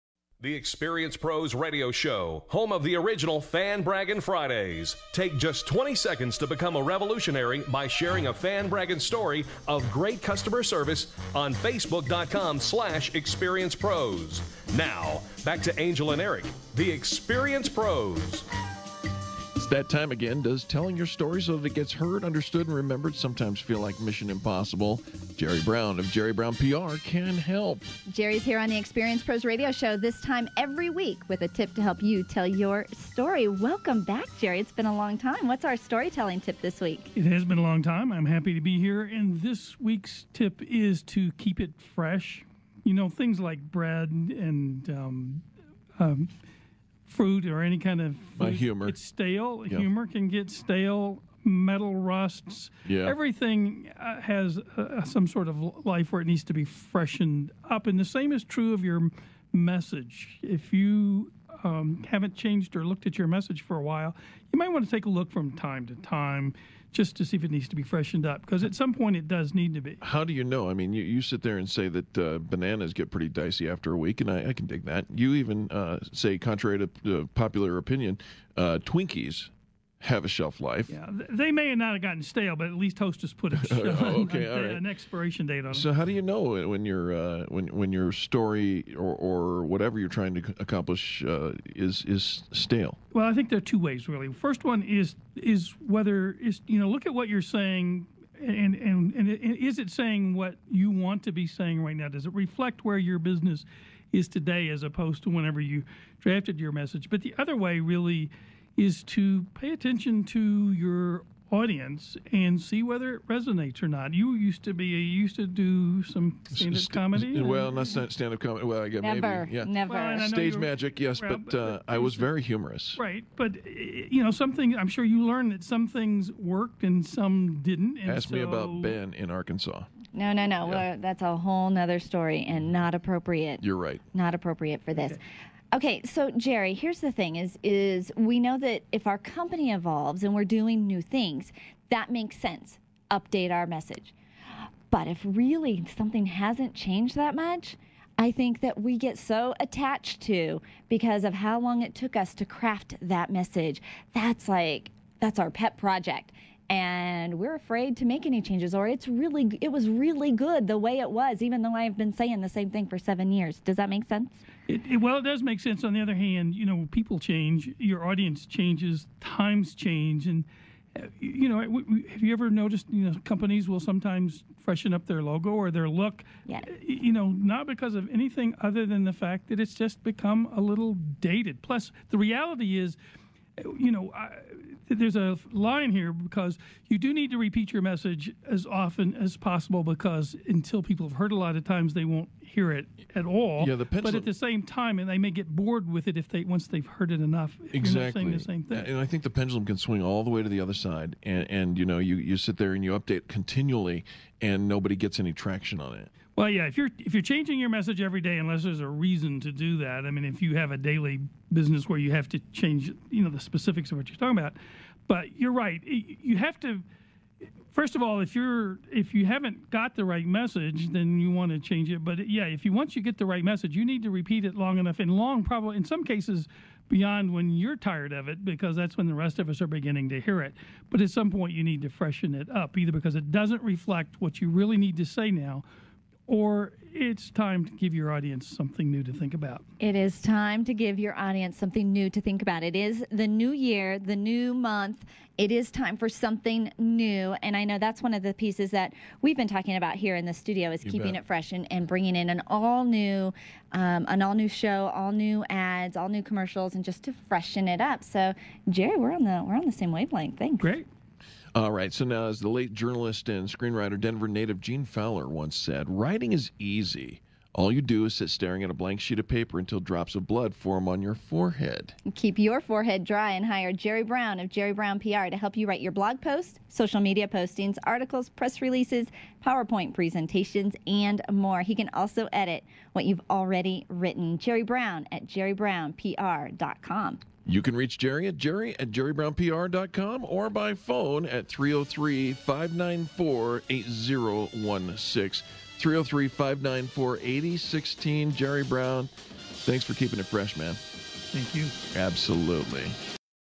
Storytelling Tips | Keep it Fresh
Has your message gotten stale? Then it's time to freshen it up. From JerryBrownPR's weekly storytelling tips on the Experience Pros Radio Show.